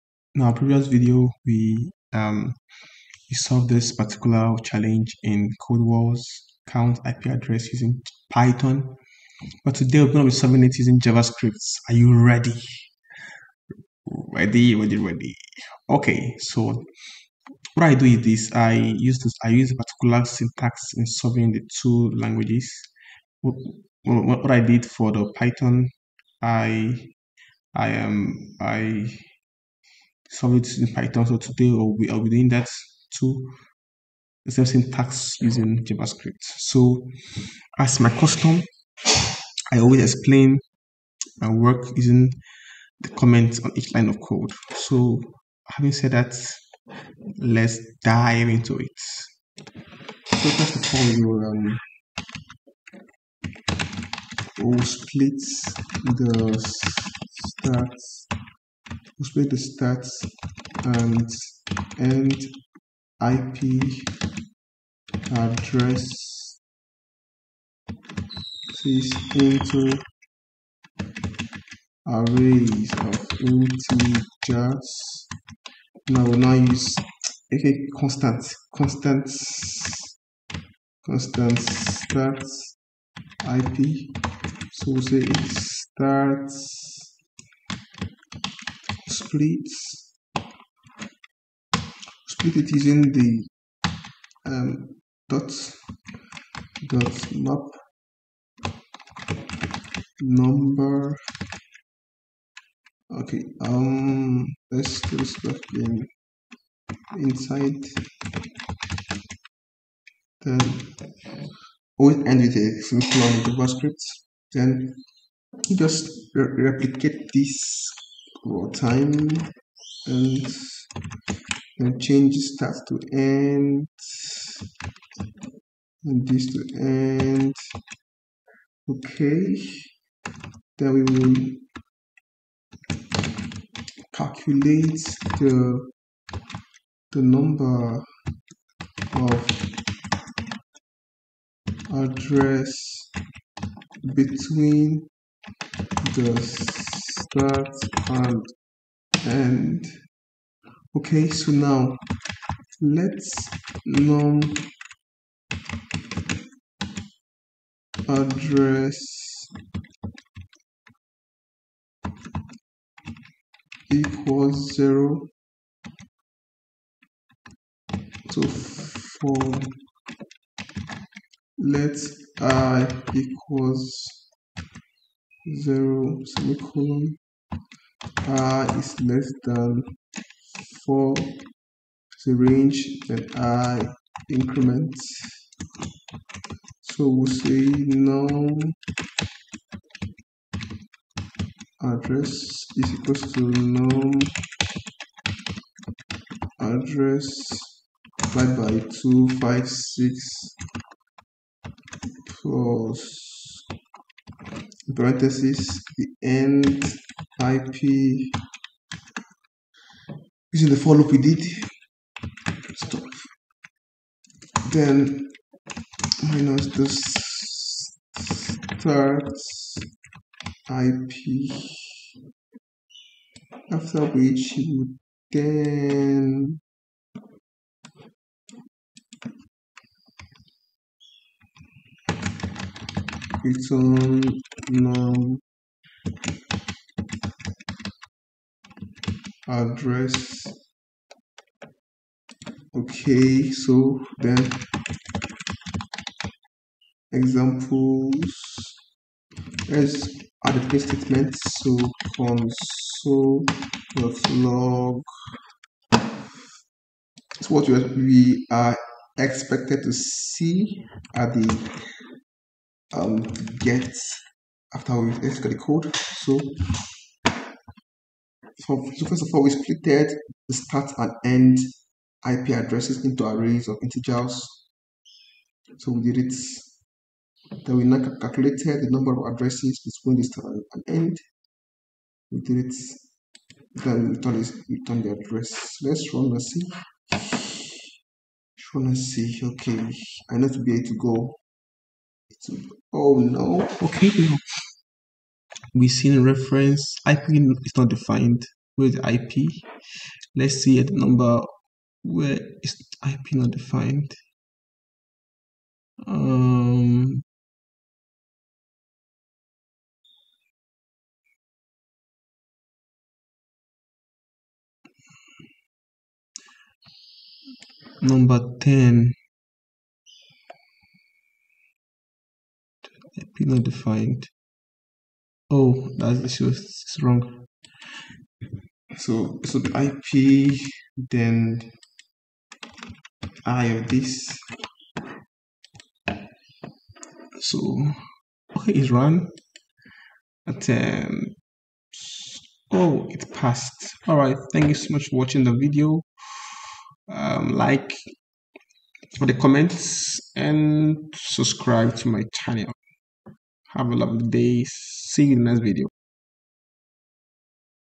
JavaScript Mastery: Count IP Address CodeWars Challenge Solution using JavaScript Programming
_Description:_ "Take your JavaScript skills to the next level! I'm excited to share my solution to the Count IP Address CodeWars Challenge, where I successfully tackled this challenging problem using JavaScript programming. In this video, I'll walk you through my thought process, coding strategy, and final solution.

Whether you're a seasoned developer or just starting out, you'll learn valuable techniques and best practices for solving programming challenges. Ready to level up your coding skills? I'm open to working with individuals and teams to solve real-world problems and drive innovation.